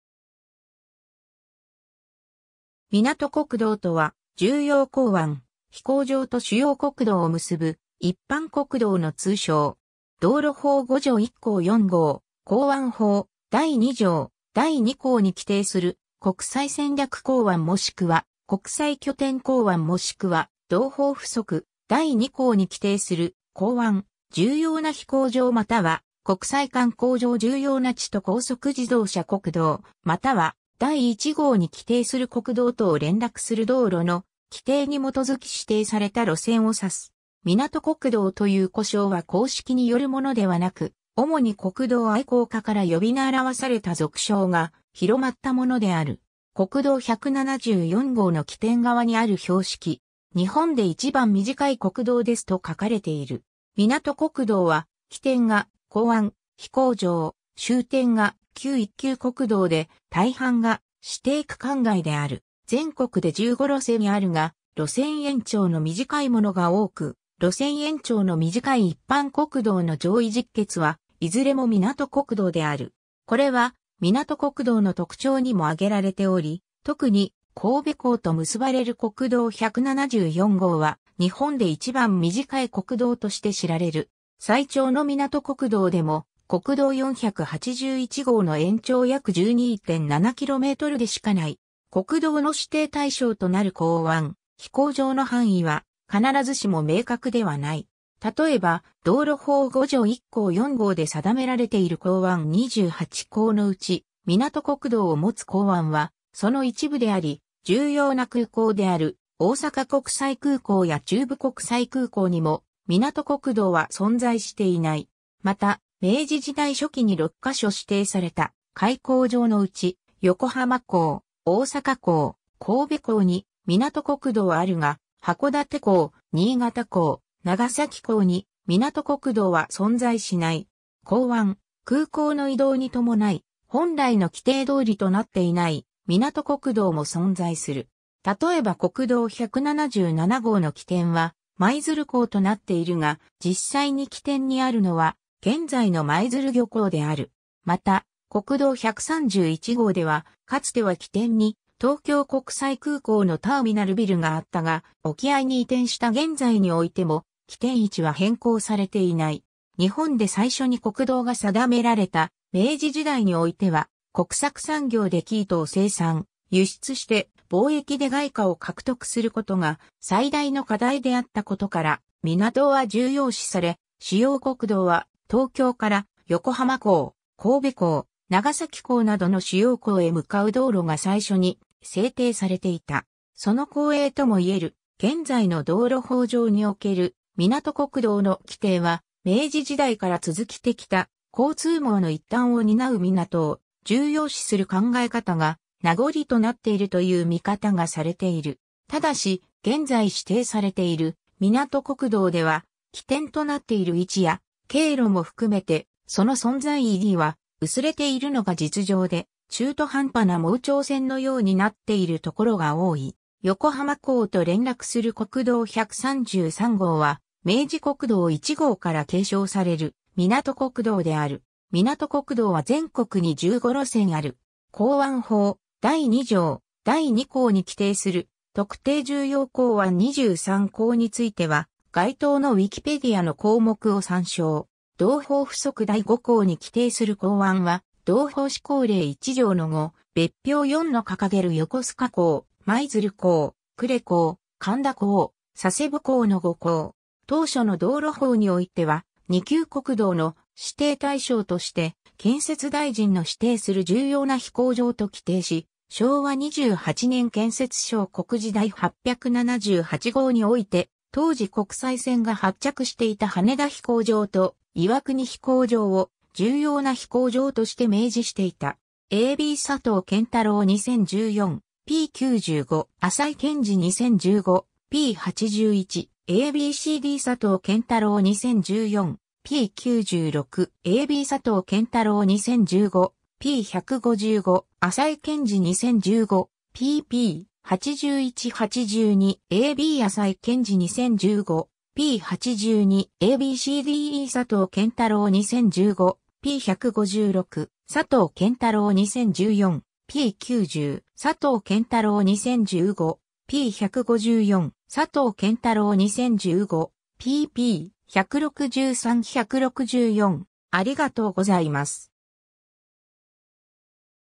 港国道
港国道（みなとこくどう）とは、重要港湾・飛行場と主要国道を結ぶ一般国道の通称。
 道路法5条1項4号「港湾法（昭和25年法律第218号）第2条第2項に規定する国際戦略港湾若しくは国際拠点港湾若しくは同法附則第2項に規定する港湾、重要な飛行場又は国際観光上重要な地と高速自動車国道又は第1号に規定する国道とを連絡する道路」の規定に基づき指定された路線を指す。
「港国道」という呼称は公式によるものではなく、主に国道愛好家（マニア）から呼び習わされた俗称が広まったものである。
 国道174号の起点側にある標識。
「日本で一番短い国道です」と書かれている。
 港国道は、起点が港湾・飛行場、終点が旧一級国道で、大半が指定区間外（道路管理者が都道府県または政令指定都市）である。
 全国で15路線あるが、路線延長の短いものが多く、路線延長の短い一般国道の上位十傑はいずれも港国道である。
これは、港国道の特徴にもあげられており、特に神戸港と結ばれる国道174号（延長187.1メートル）は「日本で一番短い国道」として知られる。
最長の港国道でも、国道481号の延長約12.7キロメートル (km) でしかない。
 国道の指定対象となる港湾・飛行場の範囲は必ずしも明確ではない。
例えば、道路法5条1項4号で定められている港湾28港のうち、港国道を持つ港湾はその一部であり、重要な空港（旧空港整備法における第一種空港）である大阪国際空港（伊丹空港）や中部国際空港にも港国道は存在していない。
また、明治時代初期に6か所指定された開港場（函館・横浜・新潟・大阪・神戸・長崎）のうち、横浜港・大阪港・神戸港に港国道はあるが、函館港・新潟港・長崎港に港国道は存在しない。
 港湾・空港の移動に伴い、本来の規定通りとなっていない港国道も存在する。
例えば国道177号の起点は舞鶴港となっているが、実際に起点にあるのは現在の舞鶴漁港（第三種漁港）である。
また国道131号では、かつては起点に東京国際空港のターミナルビルがあったが、沖合に移転した現在においても起点位置は変更されていない。
 日本で最初に国道が定められた明治時代においては、国策産業で生糸を生産・輸出して、貿易で外貨を獲得することが最大の課題であったことから港は重要視され、主要国道は東京から横浜港・神戸港・長崎港などの主要港へ向かう道路が最初に制定されていた。
その後裔ともいえる現在の道路法上における「港国道」の規定は、明治時代から続きてきた交通網の一端を担う港を重要視する考え方が名残となっているという見方がされている。
ただし、現在指定されている港国道では、起点となっている位置や経路も含めてその存在意義は薄れているのが実情で、中途半端な盲腸線のようになっているところが多い。
 横浜港と連絡する国道133号は、明治国道1号から継承される港国道である。
 港国道は全国に15路線ある。
（起点は指定対象となる港湾・飛行場） ^ 「港湾法第2条第2項に規定する特定重要港湾」23港については、該当のWikipediaの項目を参照。
「同法附則第5項に規定する港湾」は、同法施行令1条の5・別表4の掲げる横須賀港、舞鶴港、呉港、苅田港、佐世保港の5港。
 ^ 当初の道路法においては、二級国道の指定対象として「建設大臣の指定する重要な飛行場」と規定し、昭和28年建設省告示第878号において、当時国際線が発着していた羽田飛行場（現 東京国際空港）と岩国飛行場を「重要な飛行場」として明示していた。
 ^ a b 佐藤健太郎 2014, p. 95. ^ 浅井建爾 2015, p. 81. ^ a b c d 佐藤健太郎 2014, p. 96. ^ a b 佐藤健太郎 2015, p. 155. ^ 浅井建爾 2015, pp. 81–82. ^ a b 浅井建爾 2015, p. 82. ^ a b c d e 佐藤健太郎 2015, p. 156. ^ 佐藤健太郎 2014, p. 90. ^ 佐藤健太郎 2015, p. 154. ^ 佐藤健太郎 2015, pp. 163–164.